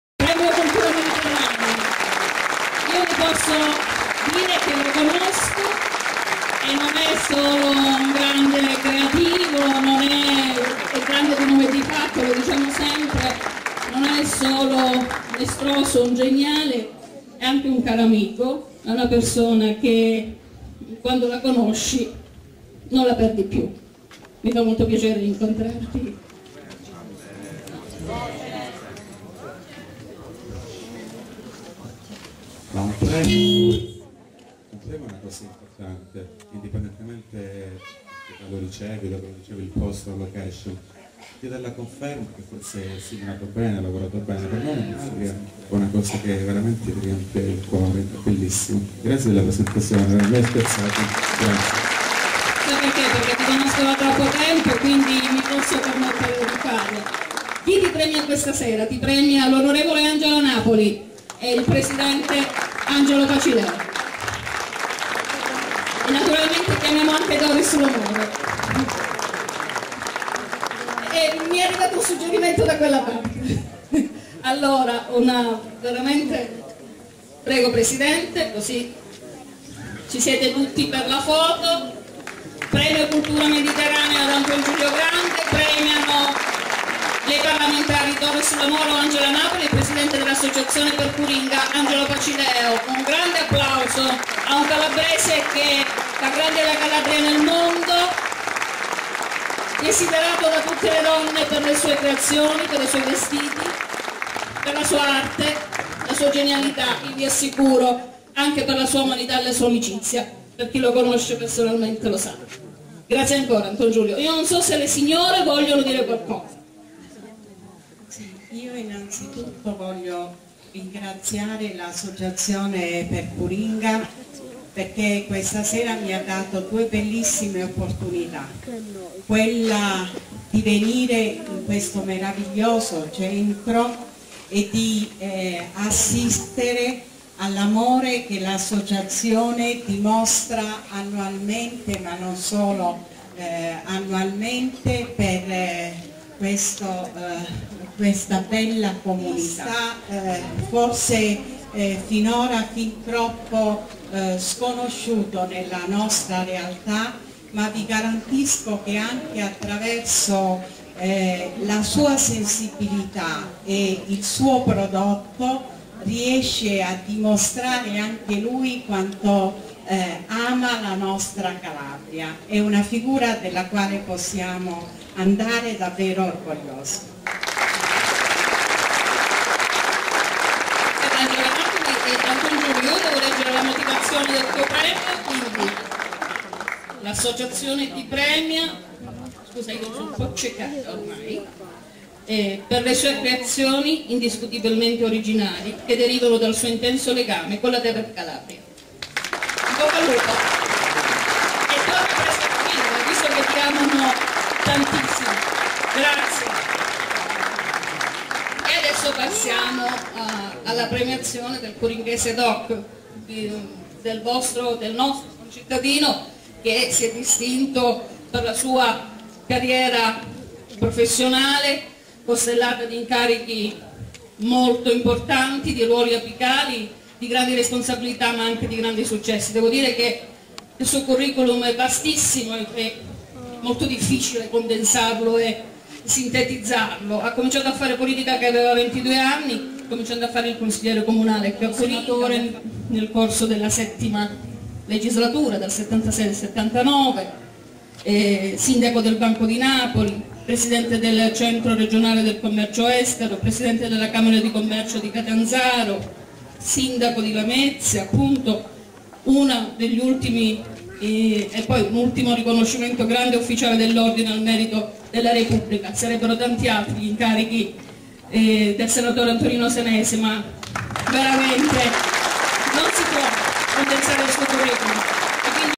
AntonGiulio GRANDE "Premio CURINGA Mediterraneo" 2012
Qui LameziaCuriosa - Lo stilista lametino AntonGiulio GRANDE riceve il "PREMIO CURINGA" del "Ventennale" dall'On. Angela NAPOLI
Franco Mirenda per LameziaCuriosa